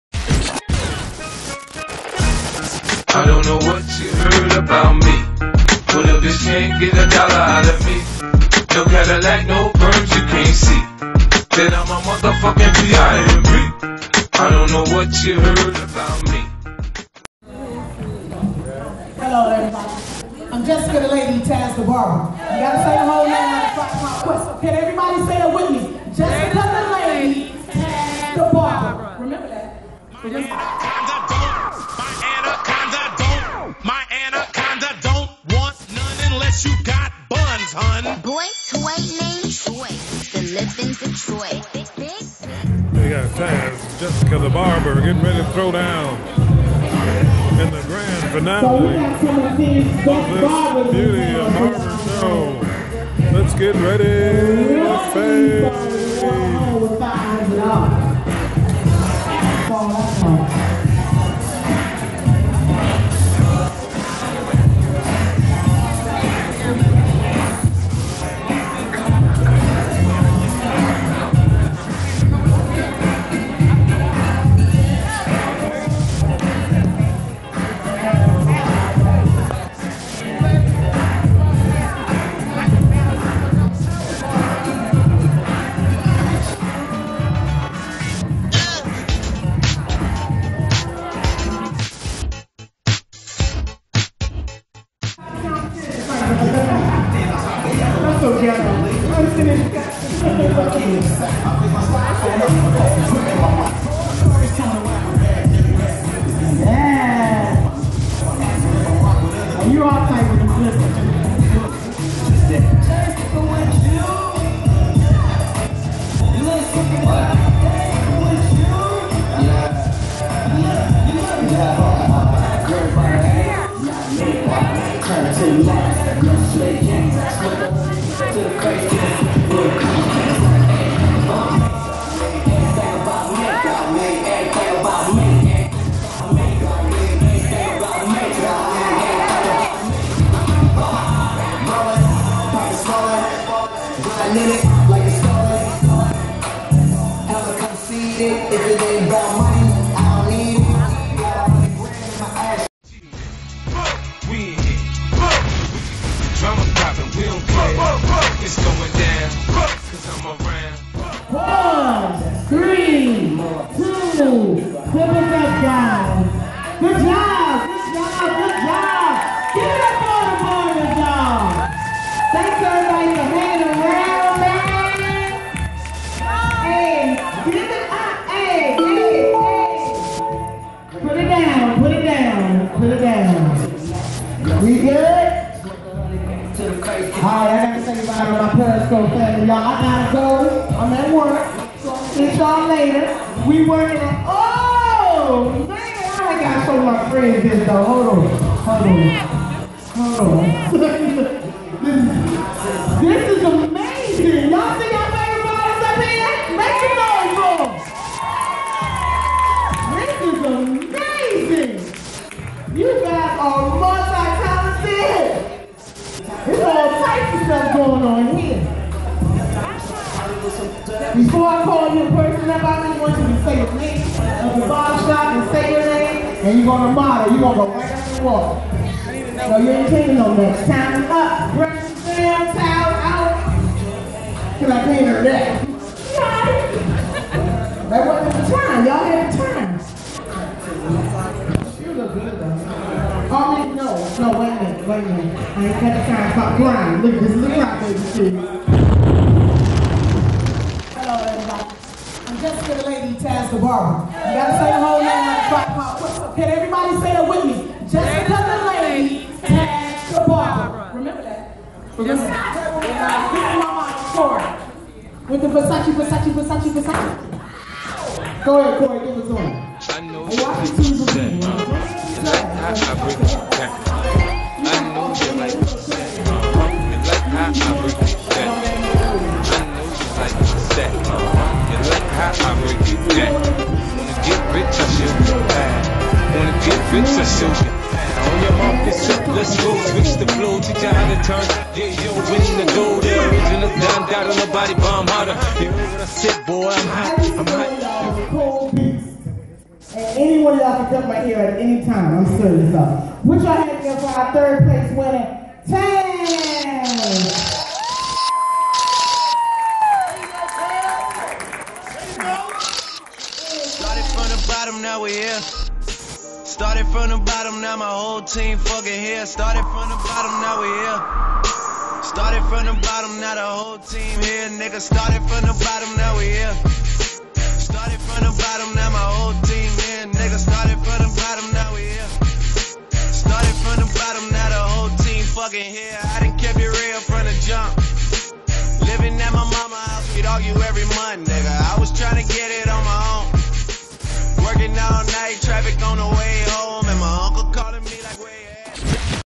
Tazzzzed the Movie! Barber Wars
Tazzzzed on the road in a barber competition